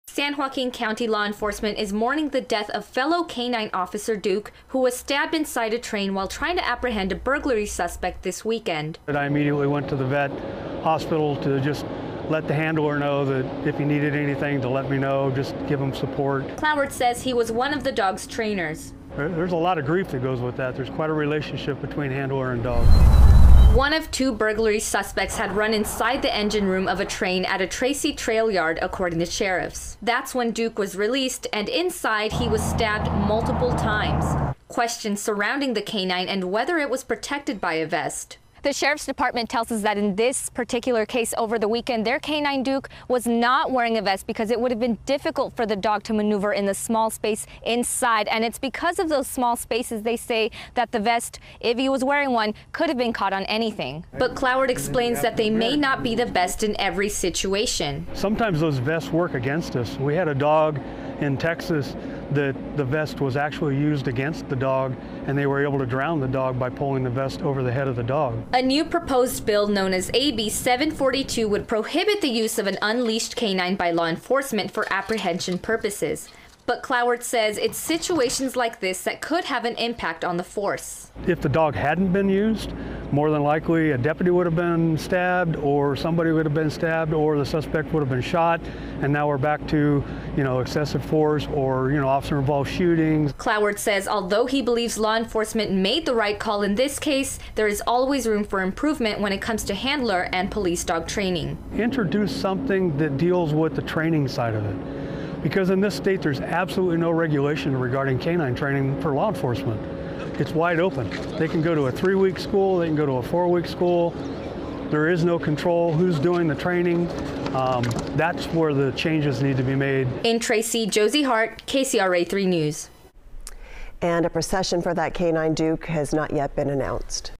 Was San Joaquin County Sheriff K-9 wearing a vest when stabbed, killed? Trainer weighs in
Members of the San Joaquin County Sheriff's Office and other local law enforcement continue to mourn the death of canine officer Duke, who was stabbed inside a train while trying to apprehend a burglary suspect this weekend.  Ron Cloward, a retired police lieutenant and canine expert, was one of Duke's first trainers. He said he immediately went to the vet hospital to offer his support to the handler.  “There’s a lot of grief that goes with that. There’s quite a relationship between handler and dog,” Cloward said.